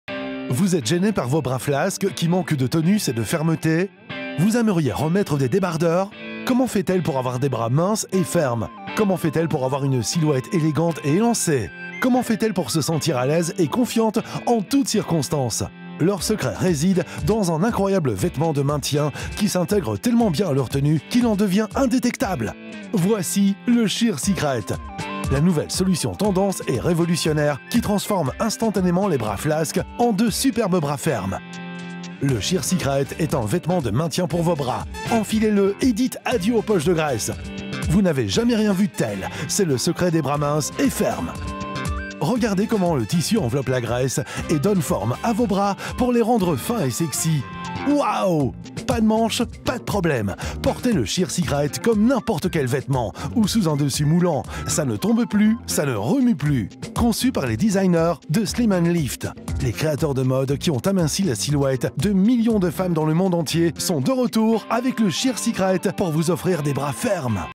Sheer Secret
Sheer Secret est LA solution révolutionnaire pour avoir des bras instantanément plus minces et plus sexy ! Il vous assurera un look plus tonique grâce auquel vous retrouverez immédiatement confiance en vous. En effet, les fibres du Slim N Lift Sheer Secret aminciront vos bras sans pour autant les compresser. De cette manière, vous paraitrez radieuse dans des robes autrefois trop moulantes ou dépourvues de manche. Vous ne pourrez bientôt plus vous passer de ce véritable accessoire de mode élégant qui vous garantira des bras fermes comme jamais ! N'attendez plus et faites confiance au savoir-faire reconnu de la marque Slim N Lift !

3 couleurs : Noir, beige et blanc
Taille : du S au XXL